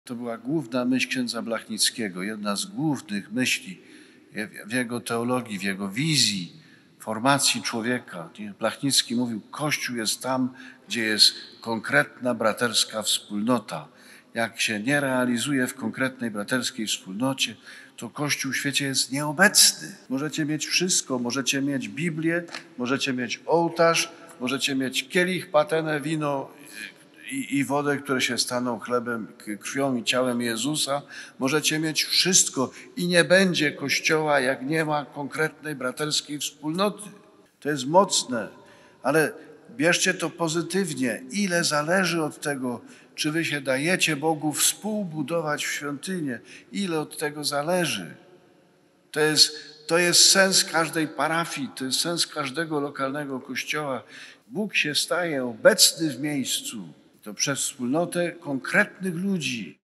Myśl braterska jest niezbędna we wspólnocie | abp Grzegorz Ryś
Abp Grzegorz Ryś przewodniczył 18 lutego 2023 r. Mszy świętej z okazji 40-lecia parafii NMP Królowej Polski w Łodzi. Metropolita łódzki wygłosił też homilię.

Parafia Najświętszej Maryi Panny Królowej Polski erygował bp Józef Rozwadowski 15 lutego 1983 r. Pierwszym proboszczem był ks. Jan Szadkowski. Pierwsza parafialna Msza święta została odprawiona 20 lutego 1983 r. przy ołtarzu polowym przy ul. Tarninowej. 13 września 1992 abp Władysław Ziółek odprawił pierwszą Mszę św. w budowanym kościele, połączoną z wmurowaniem kamienia węgielnego i aktu erekcyjnego. Także on dokonał 26 października 2008 r. konsekracji kościoła.

Komentowane czytania:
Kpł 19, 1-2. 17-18
1 Kor 3, 16-23
1 Kor 6, 19